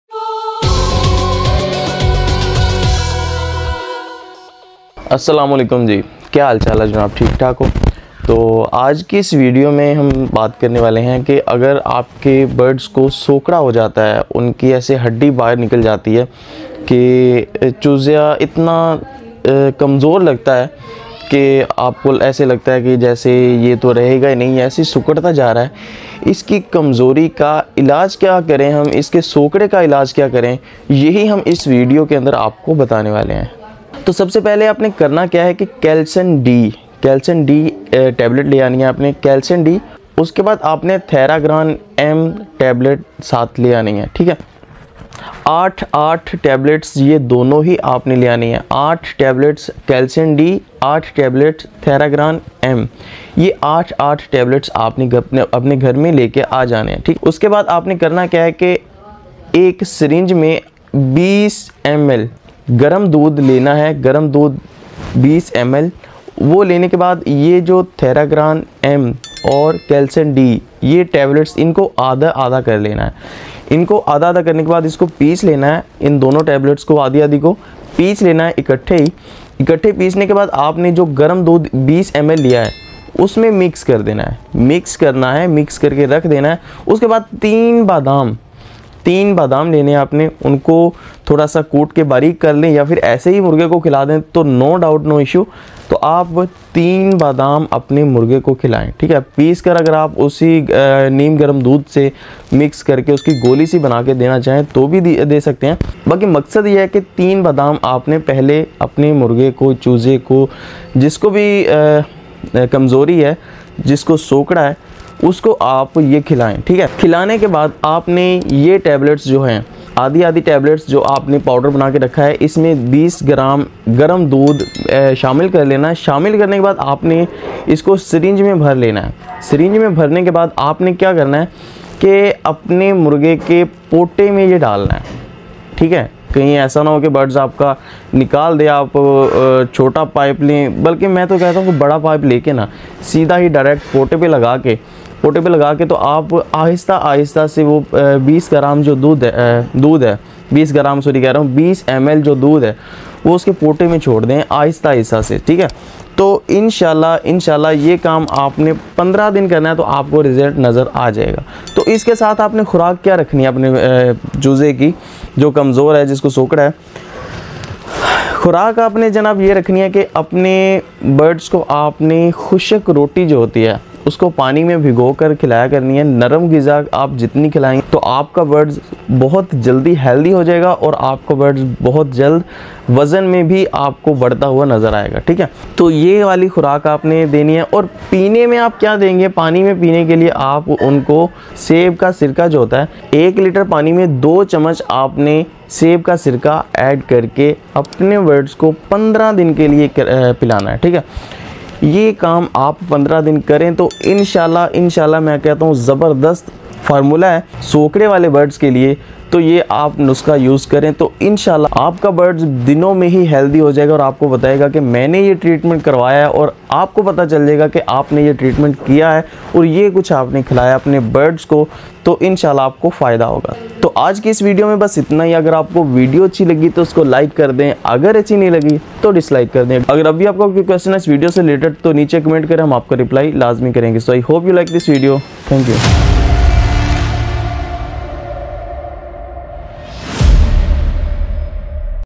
Sokra Ka ilaj By Aseel Nawabi
Zyada taqatwar aseel murghon mein sokra ka ilaj kaise karein, is video mein hum aapko sab kuch batayenge. Sokra ka ilaj ke liye mufeed nuskhe aur tajurbaat, sath hi aseel murghon ke sehatmand rehne ke tareeqay bhi aapko is video mein milenge. Dekhiye aur apne aseel murghon ko sehatmand banayein.